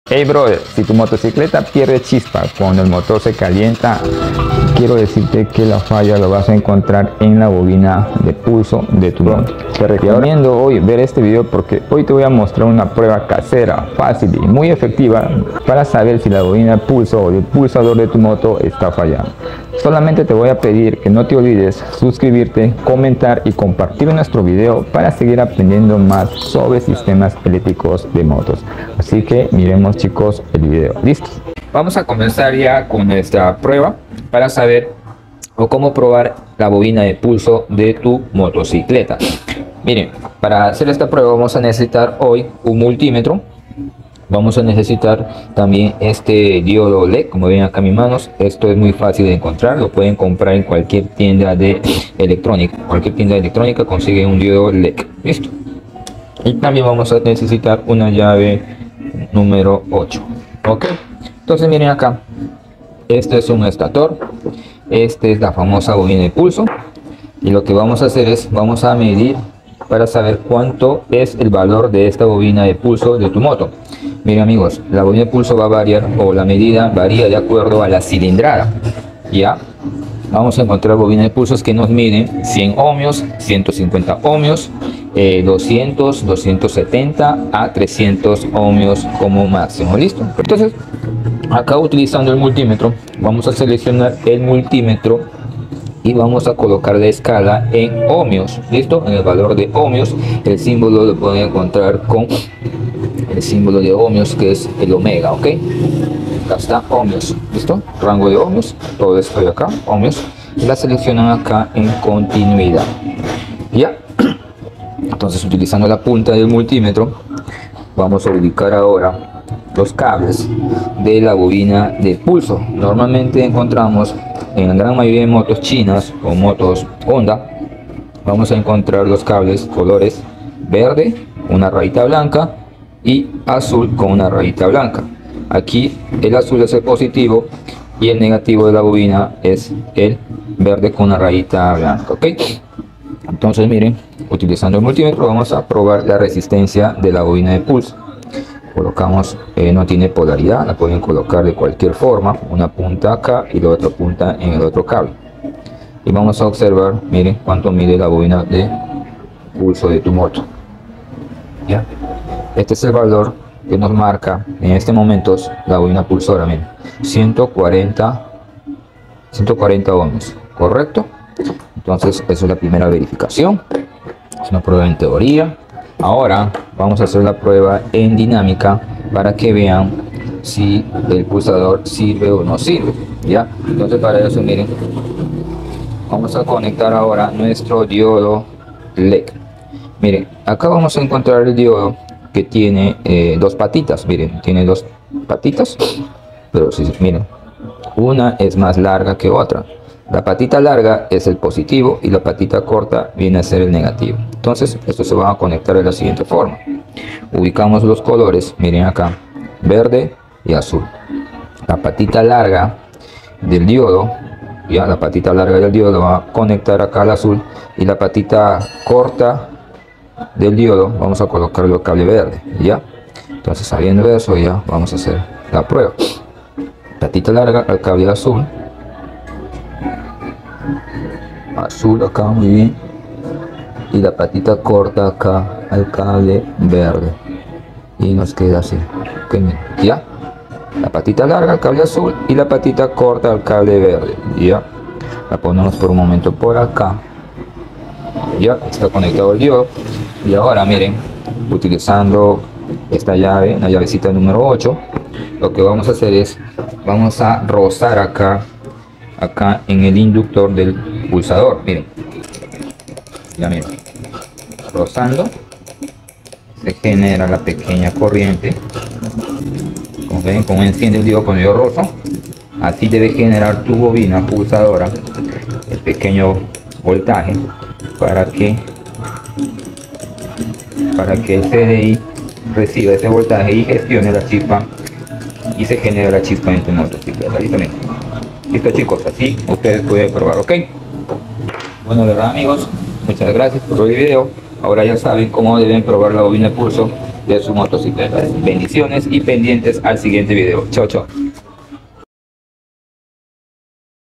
❌️ CÓMO PROBAR LA BOBINA DE PULSOS DE TU MOTO
❌️ Una prueba casera , facil y muy efectiva para saber si la bobina de pulsos esta 👍